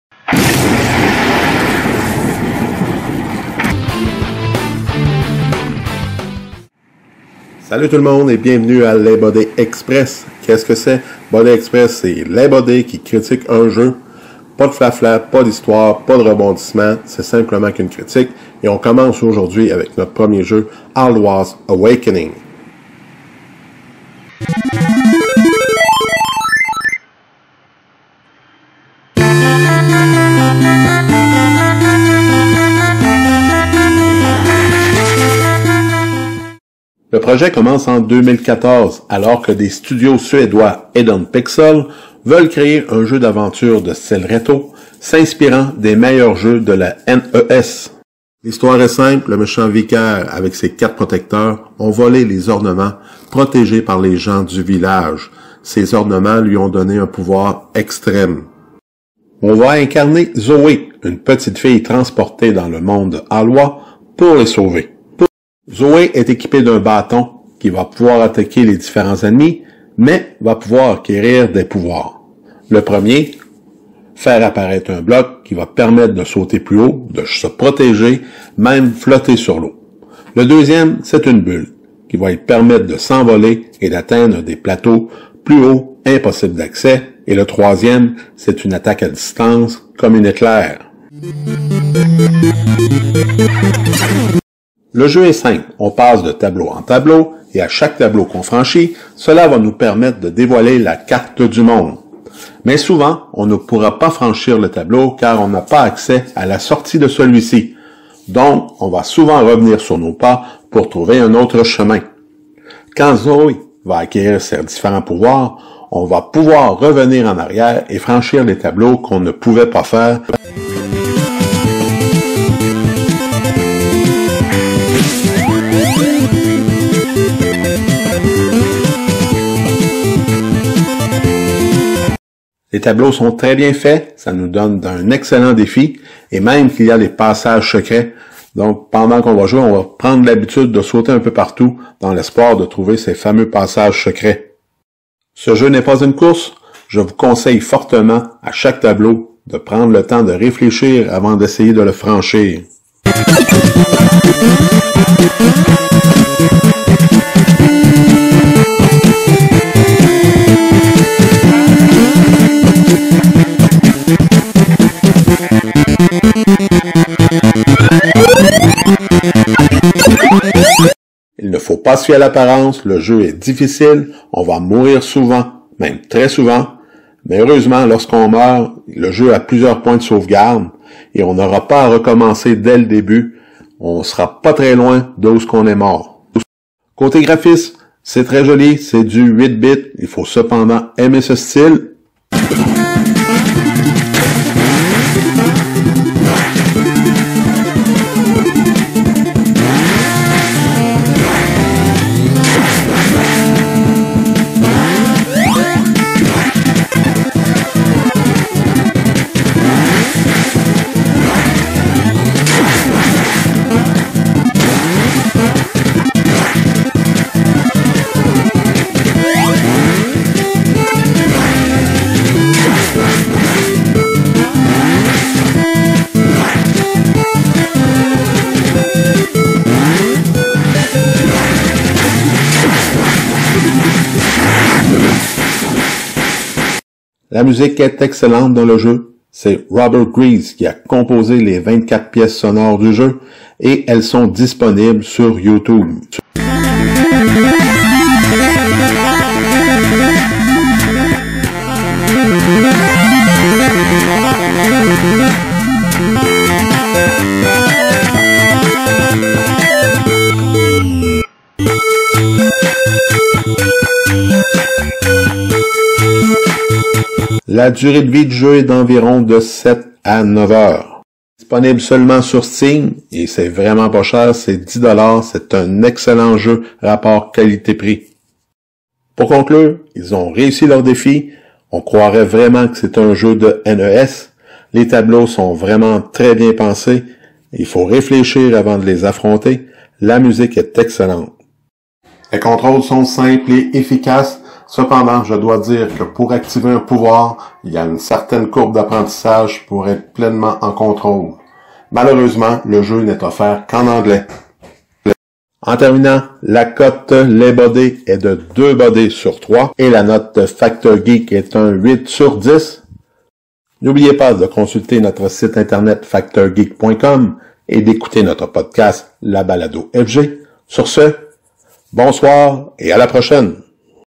Les Buddys Express: Rubrique #01 - Alwa's Awakening
Dans cette première rubrique de Les Buddys Express, Éric vous offre sa critique du jeu exclusif sur PC, Alwa's Awakening.

Il s'agit d'un jeu développé et publié par Eden Pixels. Un véritable retour à l'époque de la Nintendo NES. Les nostalgiques seront heureux. Il est disponible via Steam au coût de 10.99$.